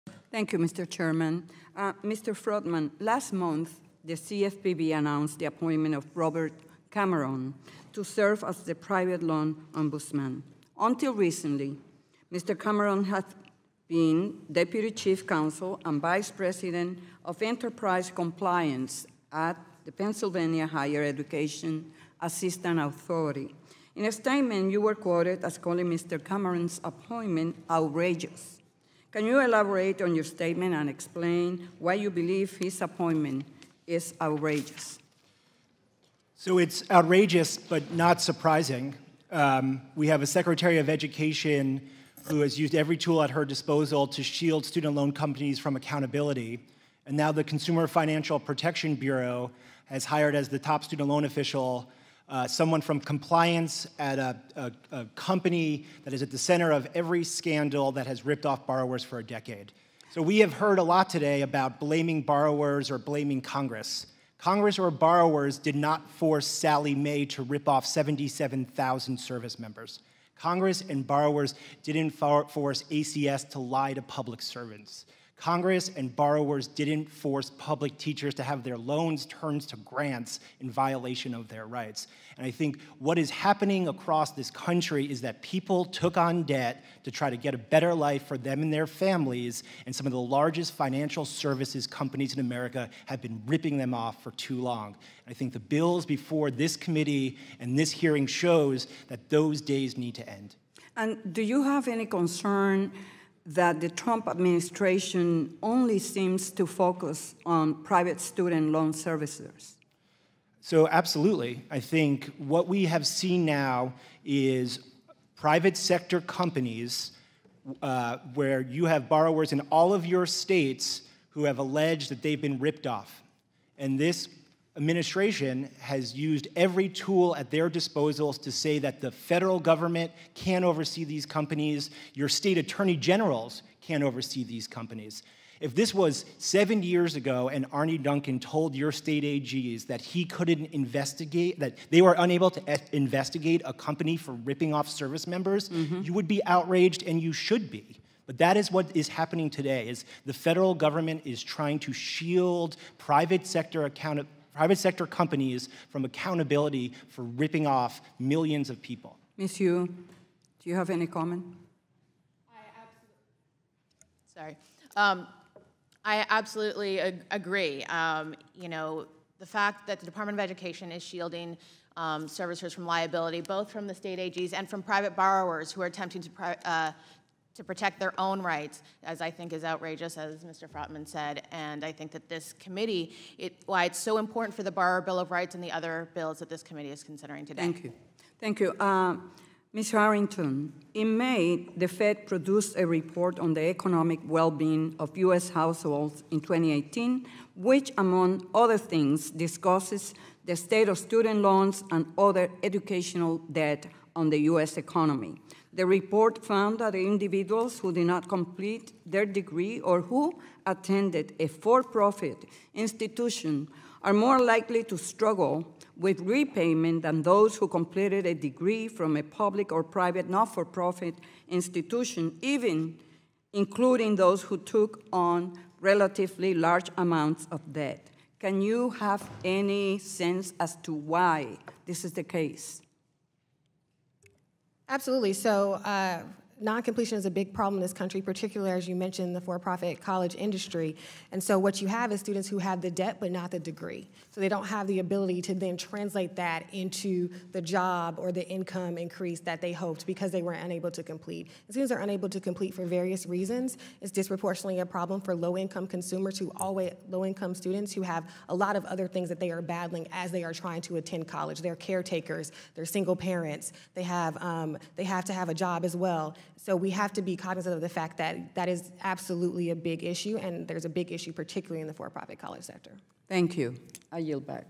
Rep. Velazquez Examines Student Loan Debt
Financial Services Committee Hearing on Student Loan Debt Crisis